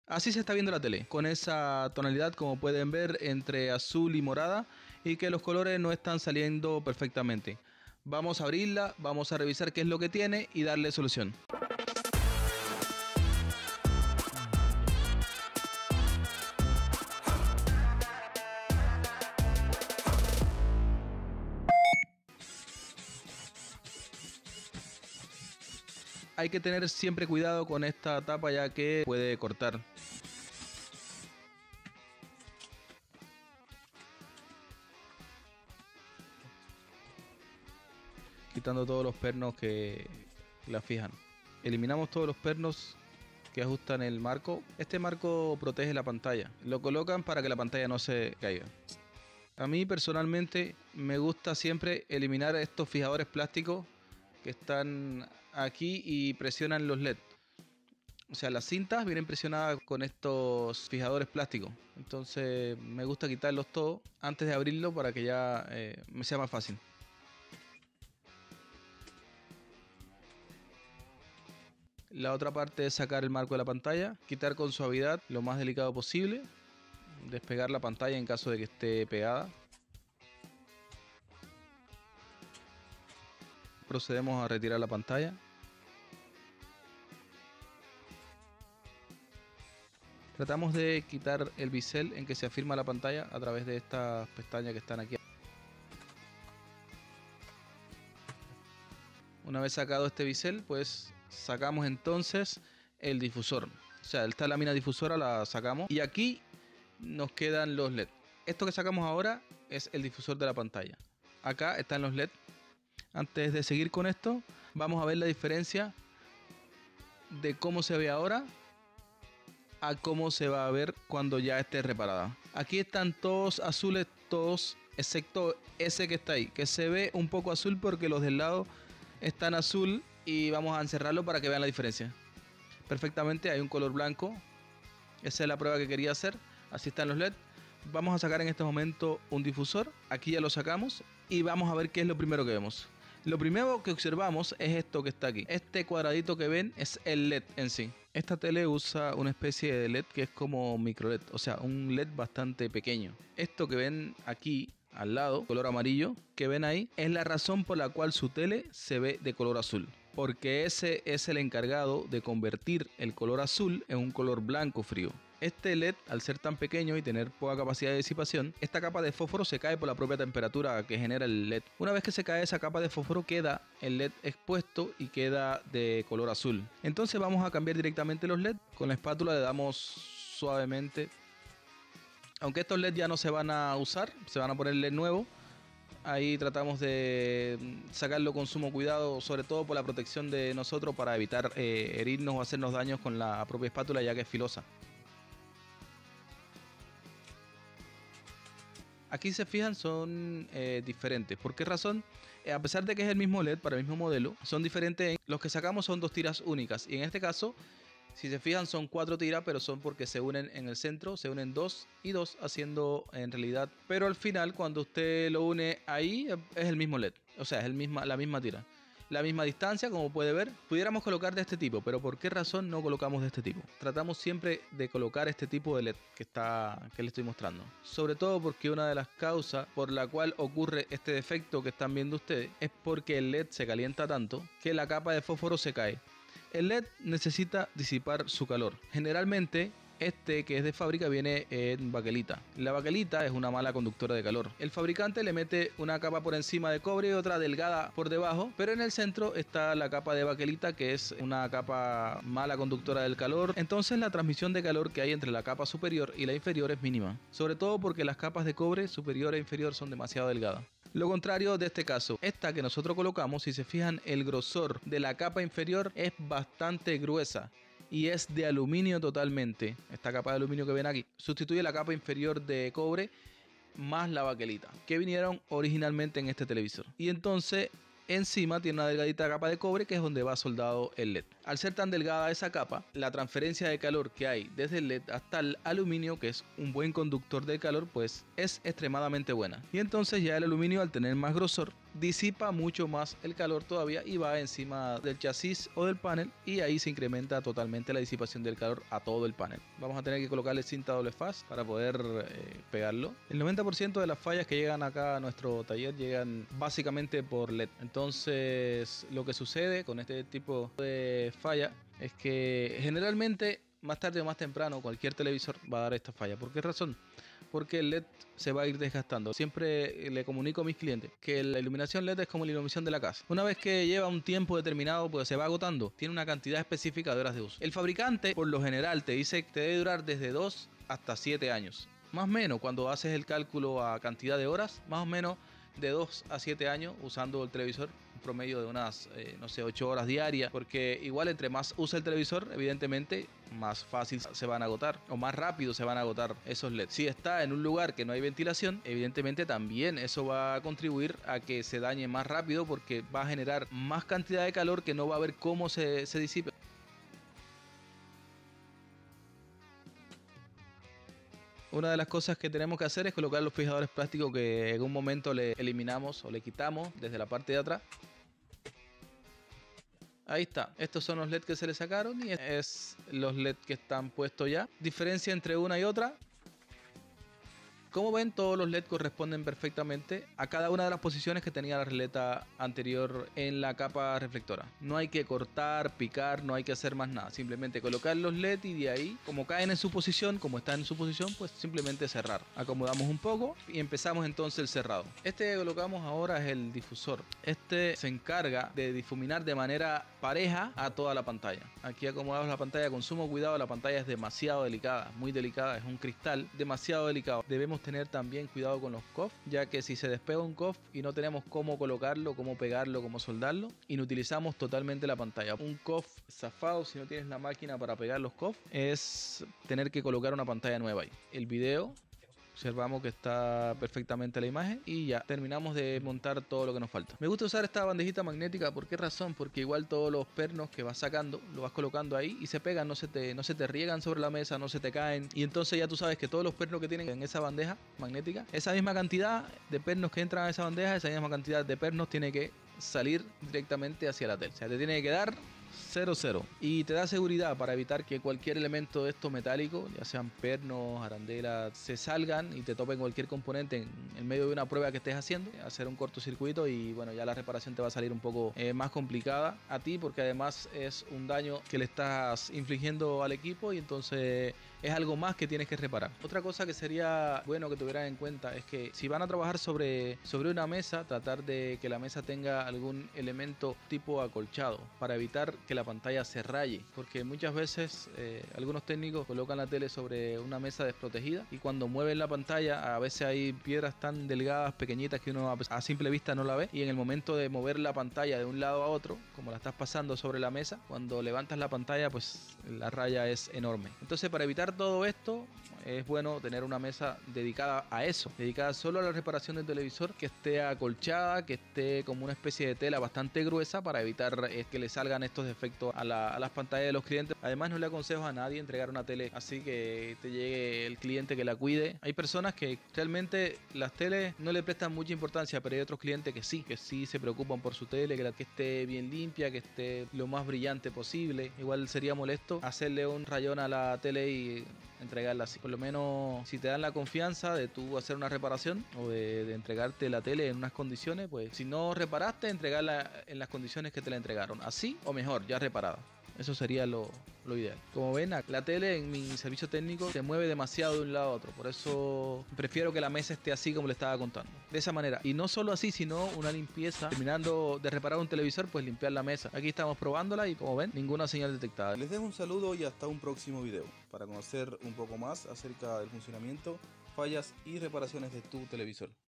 Por qué mi Televisor LG 43UJ6560, se ve Azul?
La falla de Retroiluminación es en realidad la que ocupa al menos el 80% de las fallas en los televisores modernos, la misma no siempre se manifiesta de la misma manera, aquí le traemos un caso muy particular en esta tele de la marca LG, modelo 43UJ6560, que termina su pantalla de color azul o morado como algunos clientes nos dicen. Aquí le muestro como resolver este problema y que es lo que lo causa en si.
Sígueme para que junto conmigo puedas ver, los defectos más comunes en nuestro servicio técnico y como le damos solución.